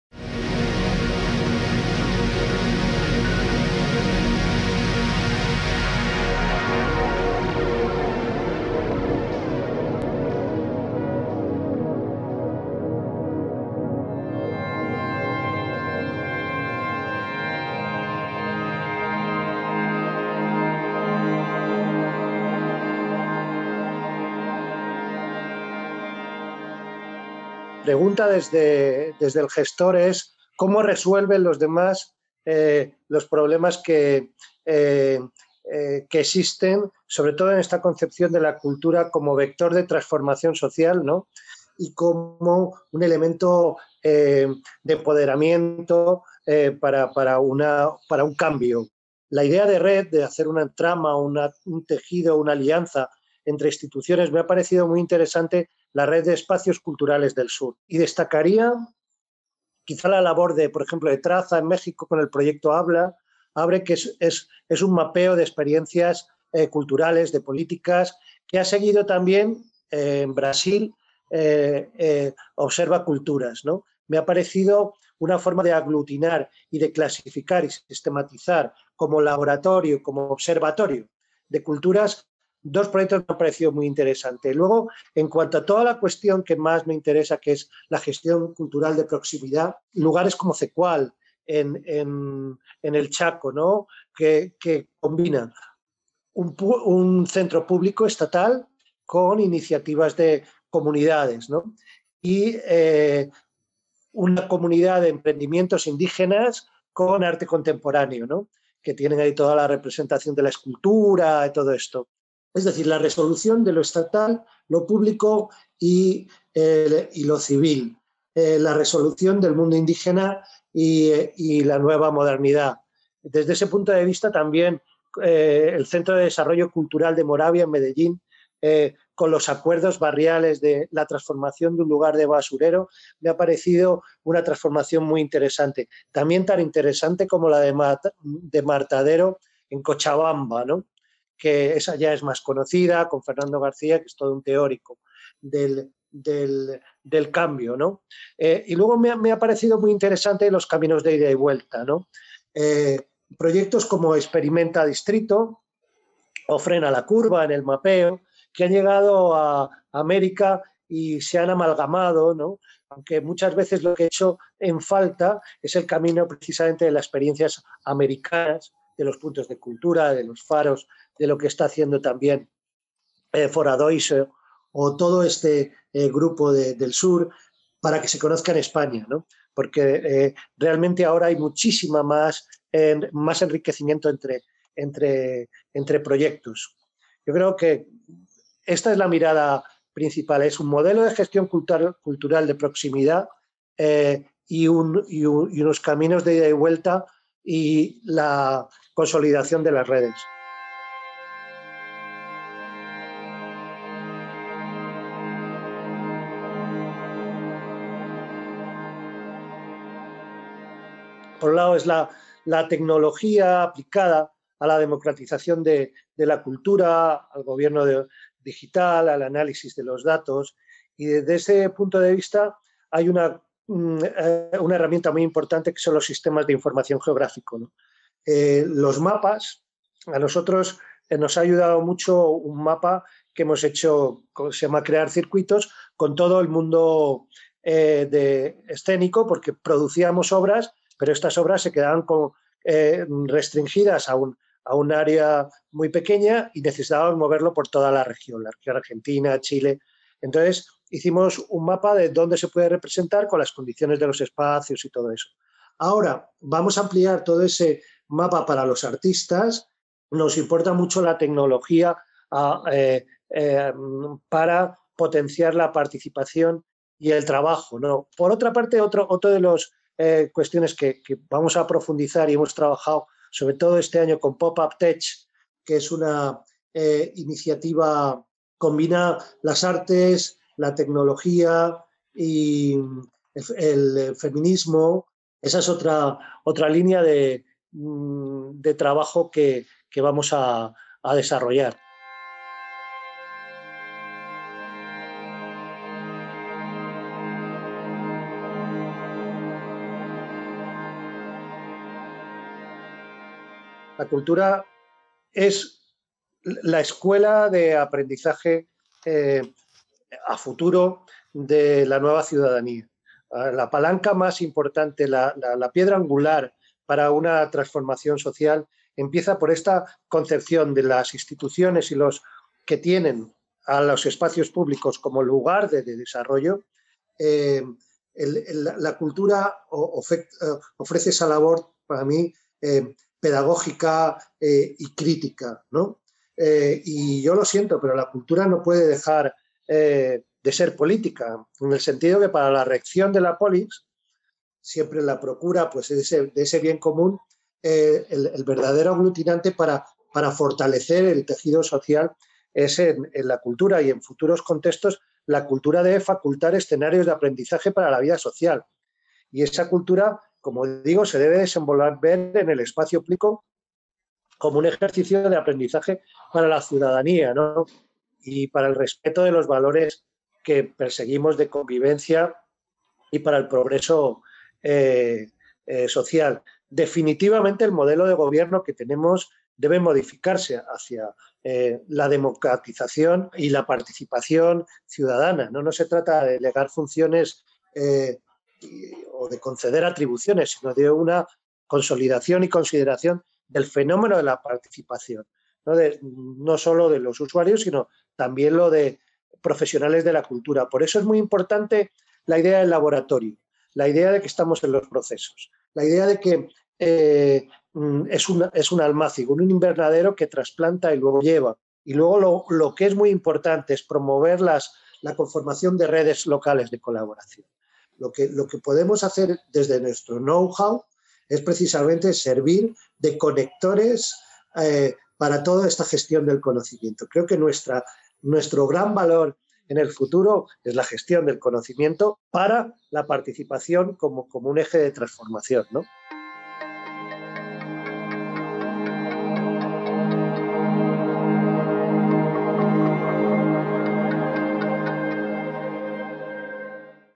Entrevista a Fernando Fajardo
VI Encuentro Cultura y Ciudadanía.
"ECOLOGÍAS. Artes, Ciencia, Tecnología, Naturaleza, Cuidados, Futuros"
11, 12 y 13 de noviembre de 2020
ECOLOGÍAS DE LA INSTITUCIÓN, TRANSICIONES DE LO INSTITUCIONAL.
Entrevista a Fernando Fajardo. Director Centro Cultural de España en Asunción, AECID.